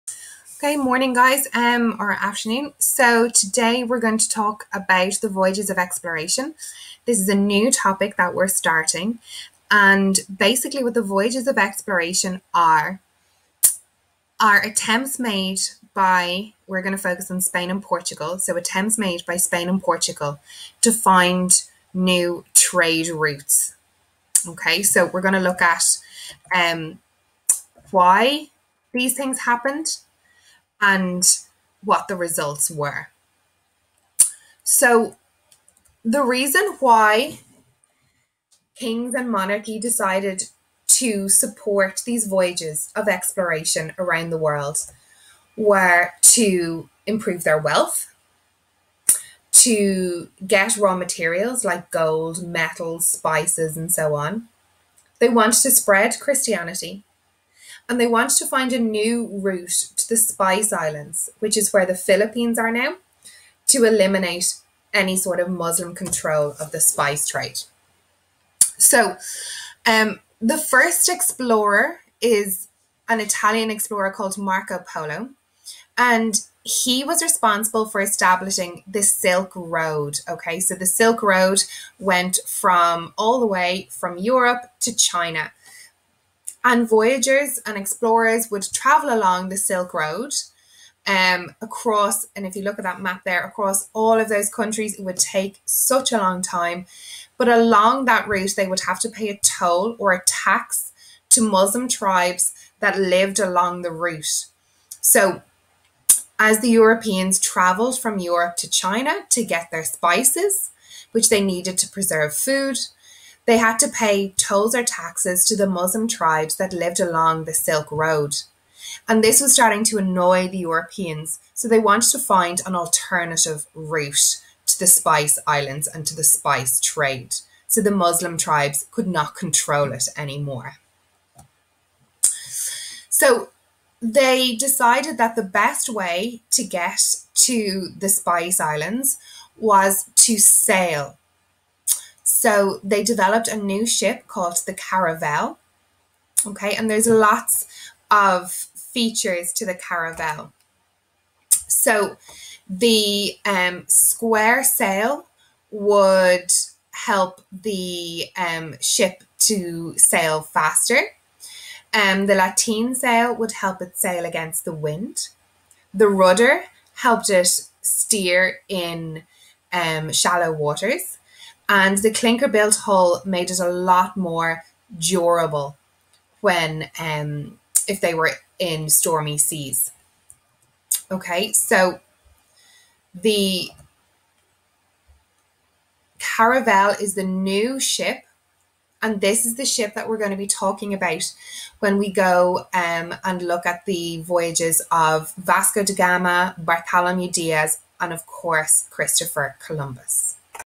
Voyages of Exploration 1
1. Reasons for the support for the voyages
2. The Sillk Road
3. The Caravel